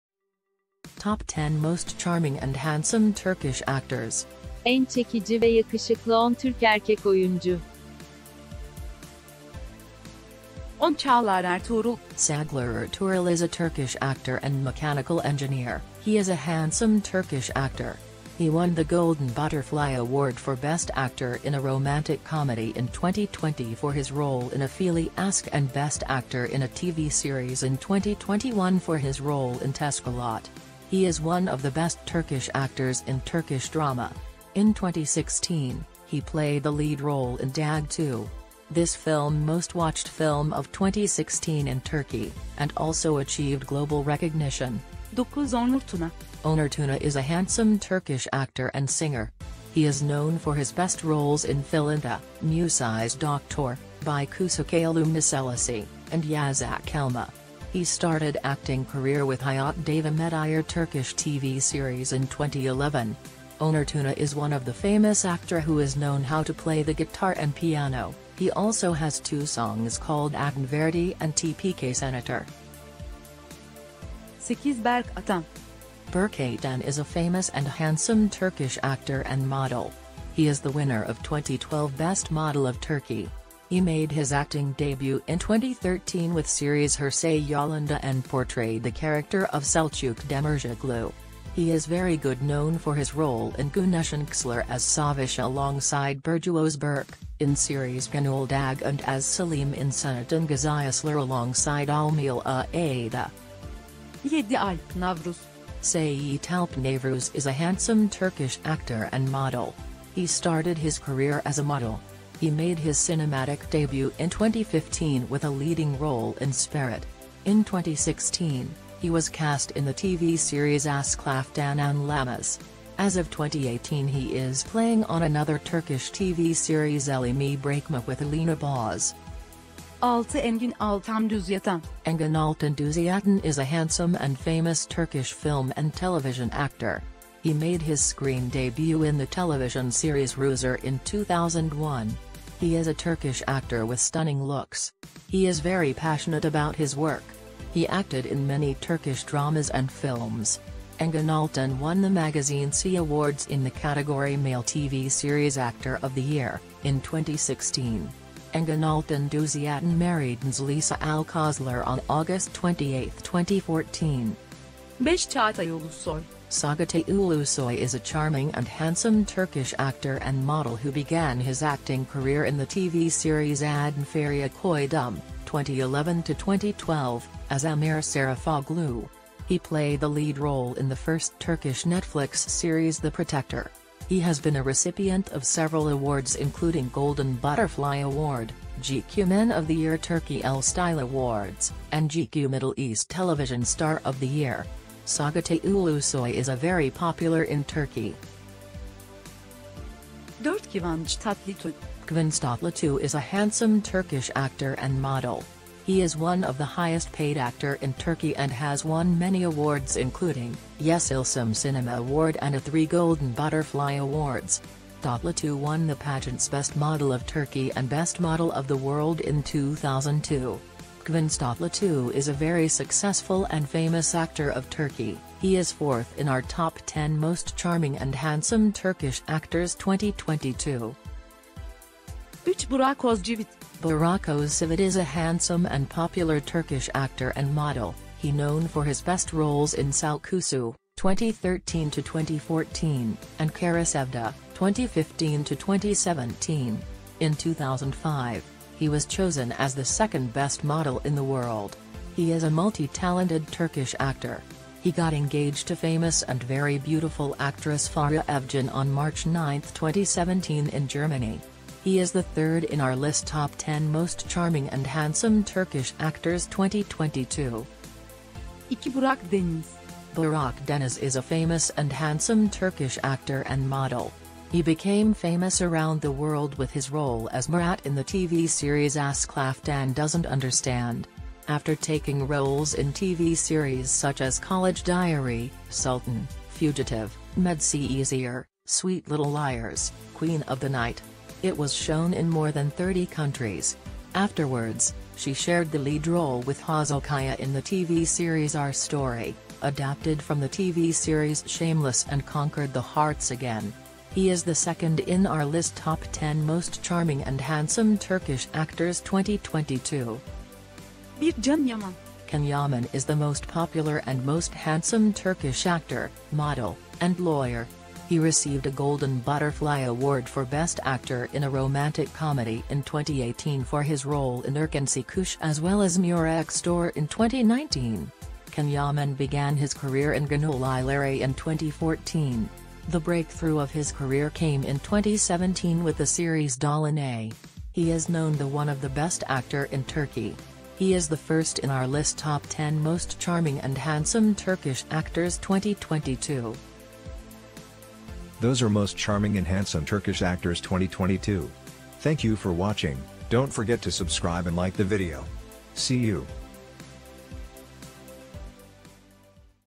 Top 10 Most Charming and Handsome Turkish Actors 2022
Turkish actors are well-known for their attractive looks, fit bodies, and success. There are many gorgeous actors in Turkey, but we've selected the Top 10 Most Charming and Handsome Turkish Actors for 2022. Can Yaman, Burak Deniz, actress Hande Ercel, and Hazel Kaya are just a few of Turkey's well-known actors and actresses. They are all multitalented, with excellent acting, modeling, and other abilities.
Take a look at our Top 10 Most Charming and Attractive Turkish Actors for 2022.
10. Çağlar Ertuğrul
9. Onur Tuna
8. Berk Atan
7. Alp Navruz
6. Engin Altan Düzyatan
5. Çağatay Ulusoy
4. Kivanc Tatlitug
3. Burak Ozcivit
2. Burak Deniz
1. Can Yaman